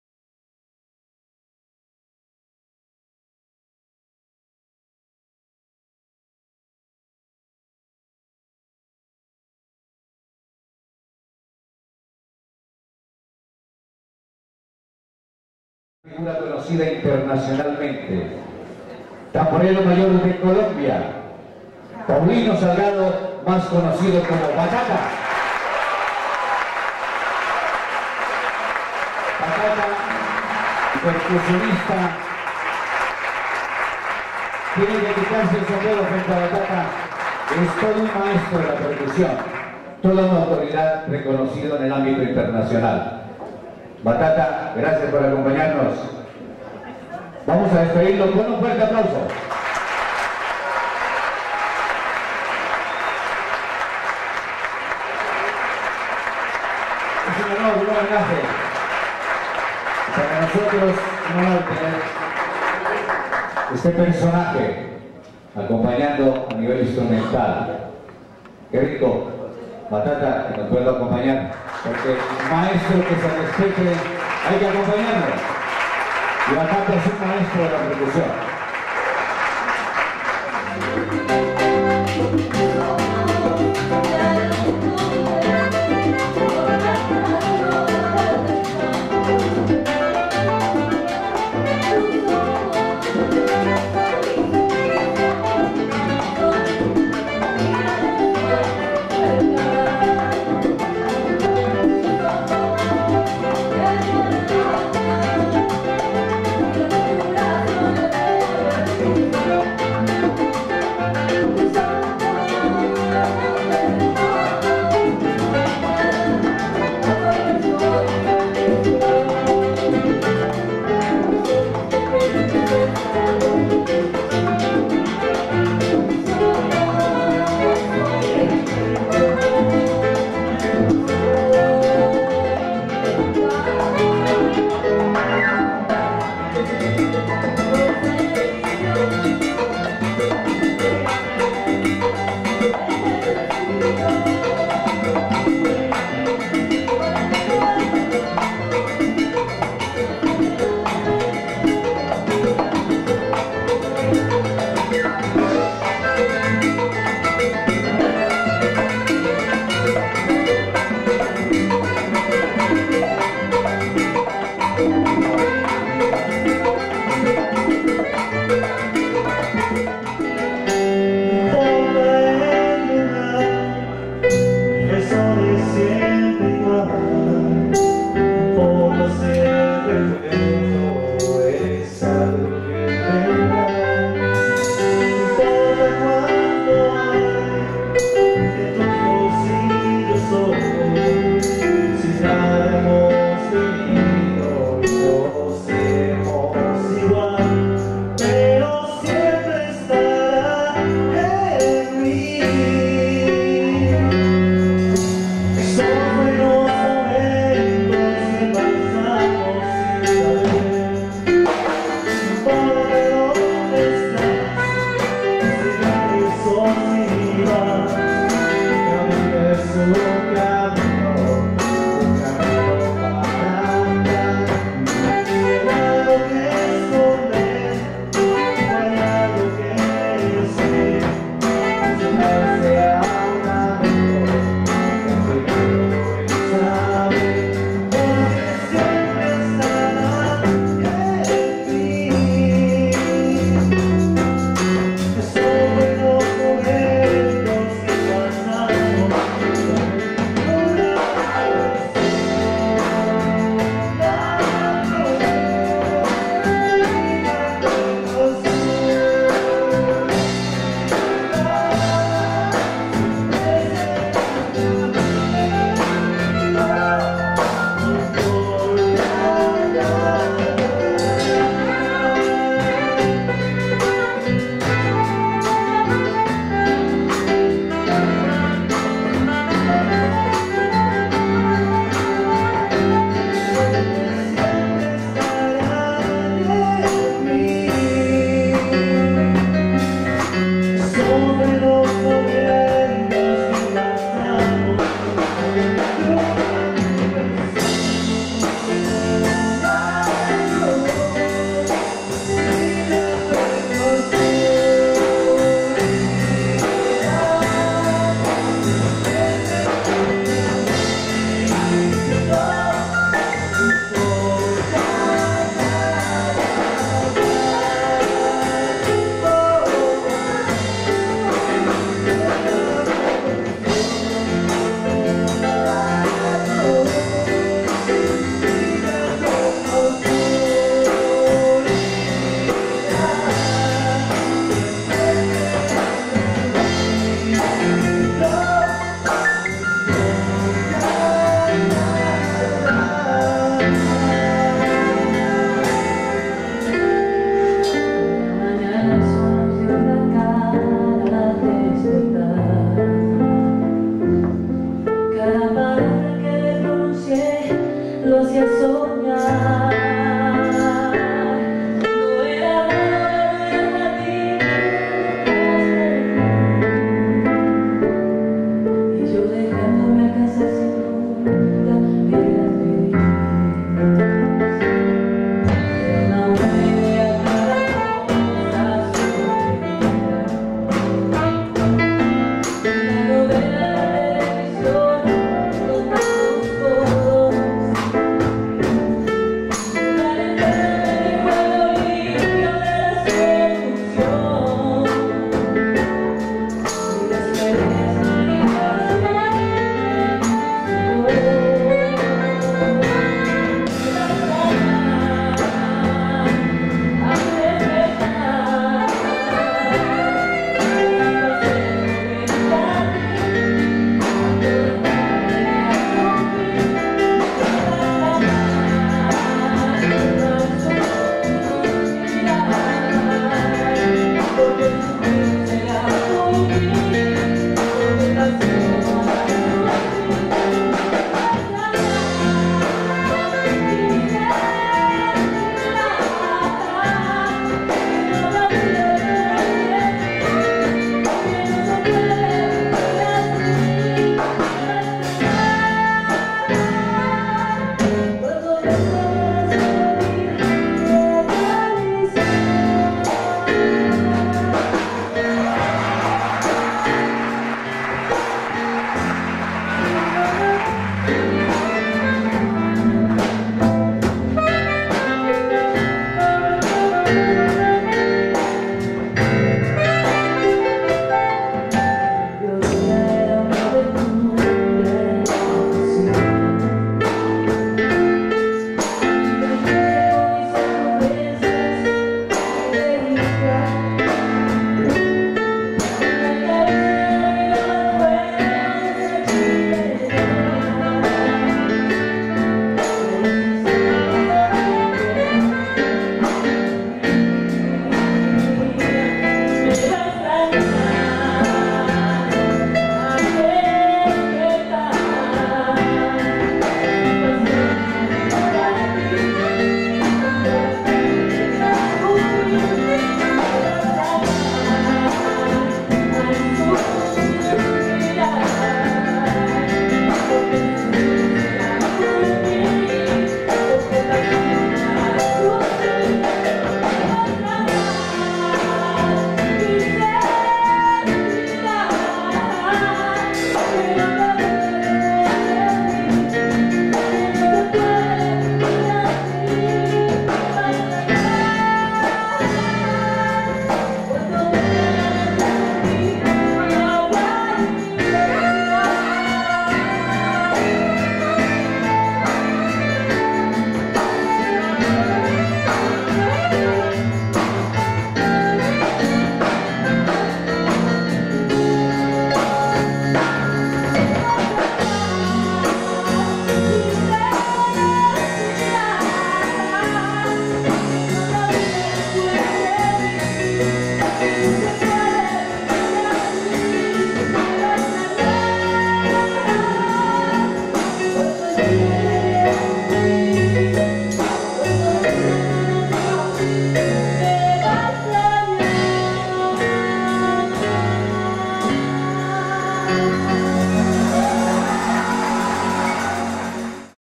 Festival Calatrava 2003: IV Parte
Imágenes del Festival de la canción Gran Cruz de Calatrava realizado en la Universidad Santo Tomás en la ciudad de Bogotá (Colombia). Participaron estudiantes de pregrado.
En esta oportunidad contamos con la presencia del maestro Paulino Salgado Batata, recordado percusionista colombiano ya fallecido, quien nos acompañó en la interpretación del tambor alegre. Participaron como músicos acompañantes entre otros, Manuel Antonio Rodríguez en el piano, dirección y arreglos musicales, Adrián Castellar en la batería, Gustavo Angulo en la percusión, Alexander Cuesta en el piano, Rubén Alarcón en la guitarra, Dagoberto García en el saxofón alto y clarinete, Alejandro Acero en el bajo y guitarra. Participaron entre otros concursantes, María Cristina Martínez, Enis Arellano, Adriana Comas, Liliana Narváez, Nelsy Garzón,